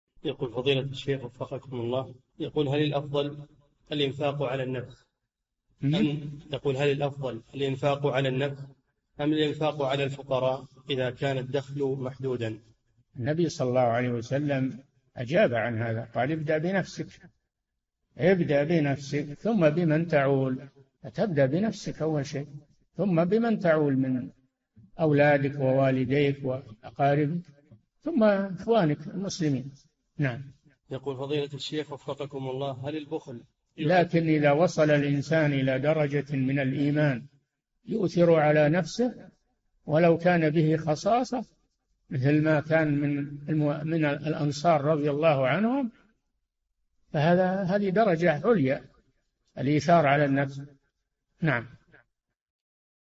[4232- 4717] إذا كان الدخل محدودا فهل الأفضل الإنفاق على النفس أم على الفقراء؟ - الشيخ صالح الفوزان
"[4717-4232] هل الأفضل الإنفاق على النفس أم الإنفاق على الفقراء إذا كان الدخل محدودًا؟
الجَوَابُ: النبي ﷺ أجاب عن هذا قال: «ابدأ بنفسك ثم بمن تعول» فابدأ بنفسك أول شيء ثم بمن تعول من أولادك ووالديك وأقاربك ثم إخوانك المسلمين، لكن إذا وصل الإنسان إلى درجة من الإيمان يؤثر على نفسه ولو كان به خصاصة مثل ما كان من الأنصار رضي الله عنهم فهذه درجة عليا، الإيثار على النفس.

للاشتراك في قناة الشيخ صالح الفوزان
فتاوى المجالس في تفسير المفصل

للاستفسار والإقتراحات والدعم
رئيس الفريق
لمشروع كبار العلماء